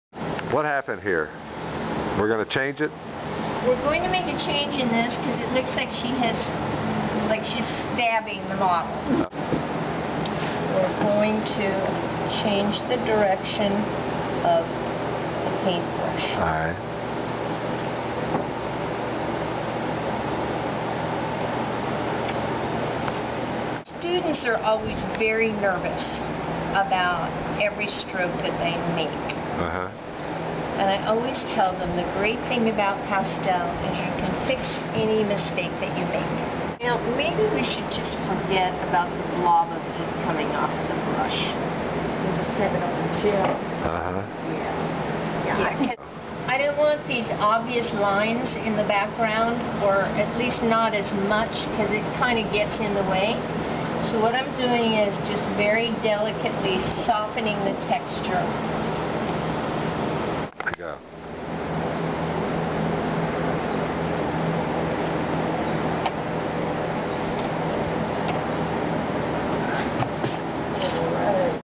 Carol Katchen Touches Up NewSixty Cover
Hot Springs artist Carol Katchen puts the finishing touches on the cover artwork she produced for the September issue of Arkansas NewSixty. In this short clip, Carol changes the direction of the paintbrush so it doesn't look like one artist is stabbing another, smooths the background and signs off.